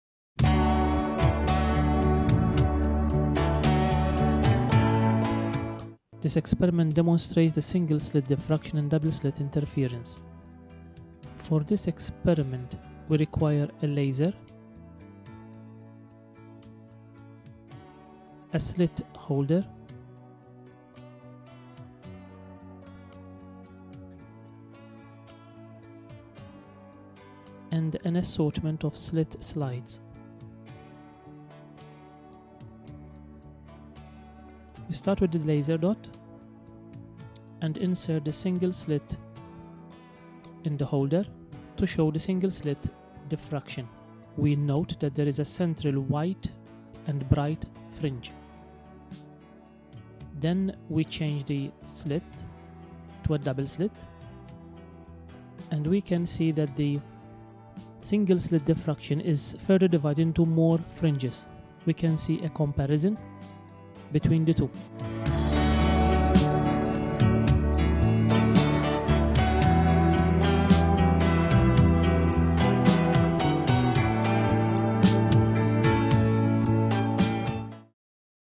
Single and Double slits
Demonstrates fringes by single slit and double slit using a laser.